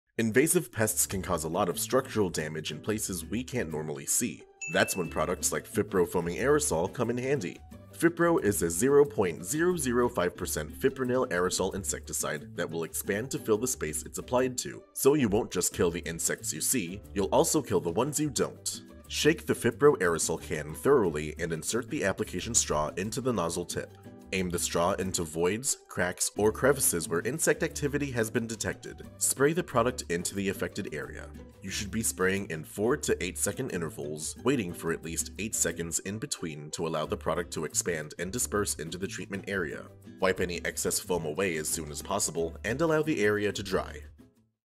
Kill Termites, Ants, & Many More W/ FiPro Foaming Aerosol!
Invasive pests can cause a lot of structural damage before we even know we have them. That’s when products like FiPro Foaming Aerosol come in handy. FiPro is a 0.005% fipronil aerosol product that will expand to fill the space it’s applied to, so you won’t just kill the insects you see, you’ll also kill the ones you don’t. This product is great to treat the voids, cracks, and crevices we can’t see in, and we primarily recommend this product to treat pests like carpenter ants, termites, and other wood-infesting insects.